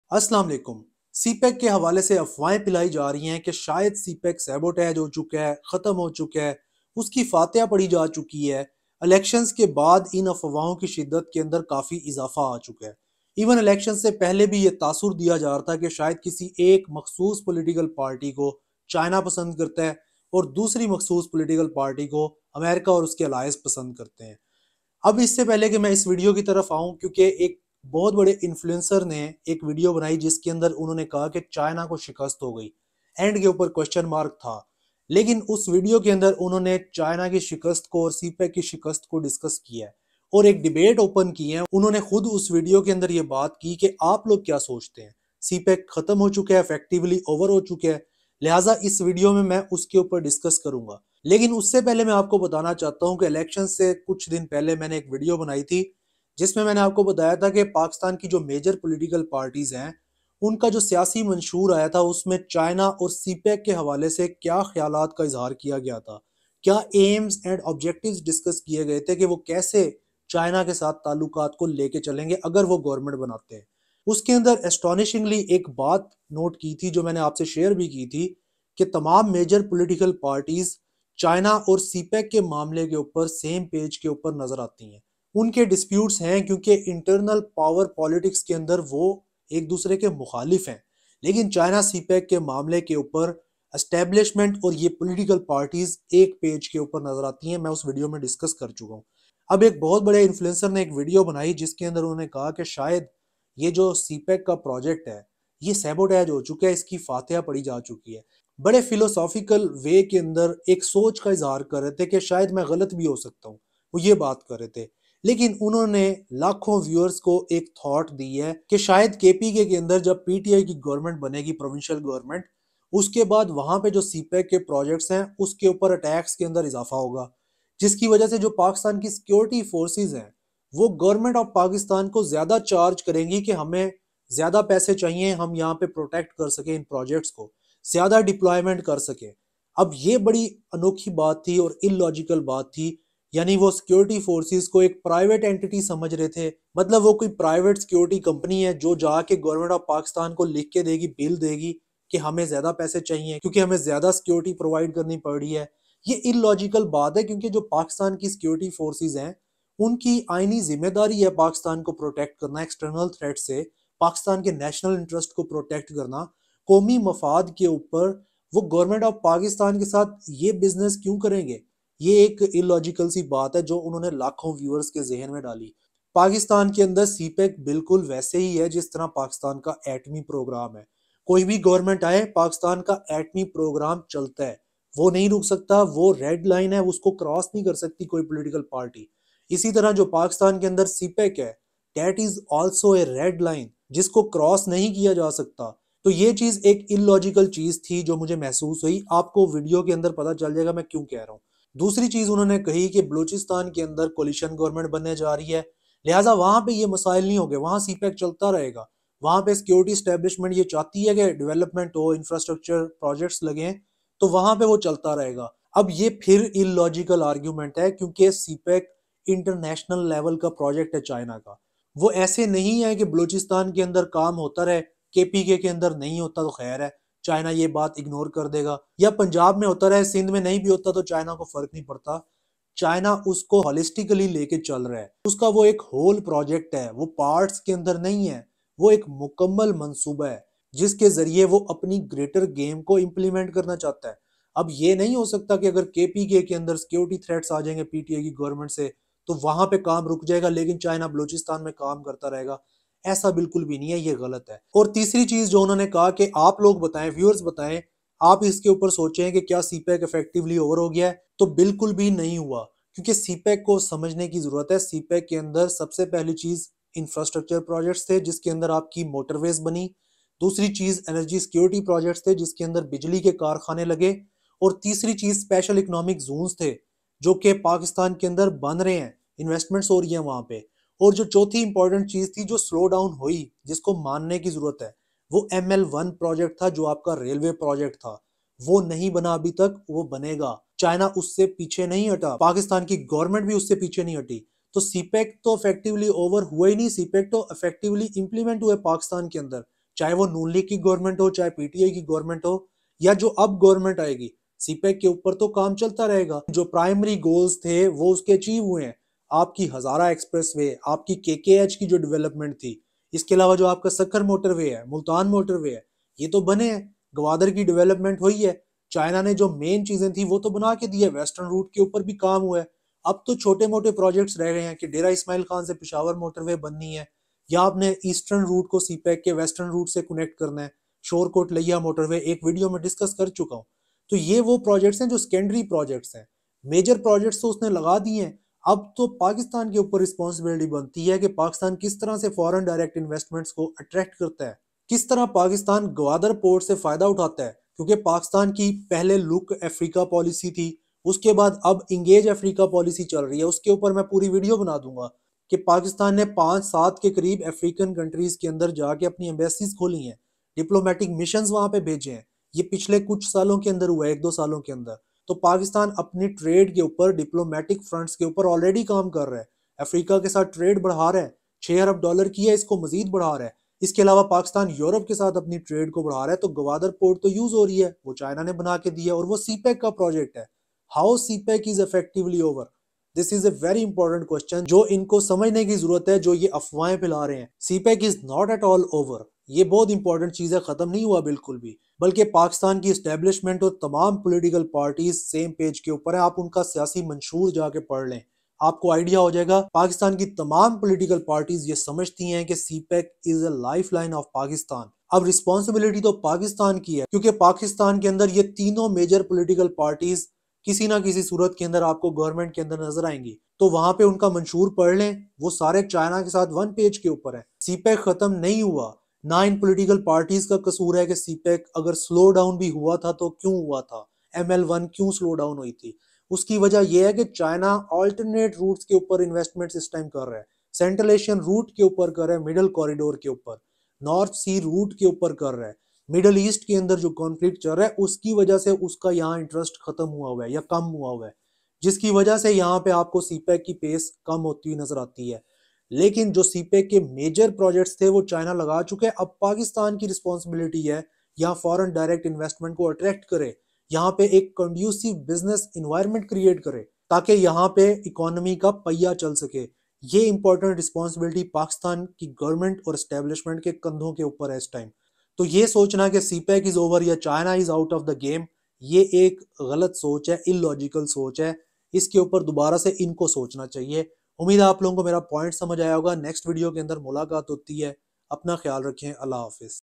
CPEC Projects CLOSED in Pakistan? | What Is The Reality?
In this video, I've discussed about the debate regarding CPEC. Some are opining that CPEC is over from Pakistan. Others, like me, argue that CPEC is very far from being over. I've discussed about recent video of Rizwan Razi on channel Razi Nama, wherein he pondered on the notion whether CPEC is over from Pakistan or not. I've discussed his points and presented a counter arguments regarding the myths suggested by him.
I've also discussed about the security of CPEC projects and held that CPEC is a national security geo-economic project of Pakistan just like the Atomic program of Pakistan which can not be politicized. I've also discussed about the details of CPEC and suggested that CPEC has been implemented in Pakistan to a greater extent, and that Pakistan must take the responsibility to further the progress in economy of Pakistan by utilizing Gwadar port, Motorways, Energy projects and Special Economic Zones.
I've also discussed that CPEC is not over because of the fact that most of its projects are operational and under good use. I've also discussed that Pakistan is using the Gwadar port, which was developed by CHINA under CPEC initiative, to engage with African nations under its "Look Africa" and Engage Africa Policy.
I've also discussed that all political parties in Pakistan which would be in the Government in Pakistan share the same vision when it comes to China and Pakistan. I've also discussed that PMLN, PTI, PPP and the security establishment of Pakistan are all on the same page regarding CPEC.
I've also stressed that the Government of Pakistan must devise the strategy to attract Foreign Direct Investment in Pakistan specially in the Special Economic Zones of Pakistan. In this way the economy of Pakistan will flourish and the benefits of CPEC can be ripped.
I've also discussed that CPEC was slowed down in the recent past, for China diverted its attention on Central Asian Trade Route, The Middle Corridor, and the North Sea Trade Route. China also diverted its energies and resources away because of the conflicts in Middle East. It has not at all abandoned CPEC.

CPEC Over?
Kya CPEC band ho gya?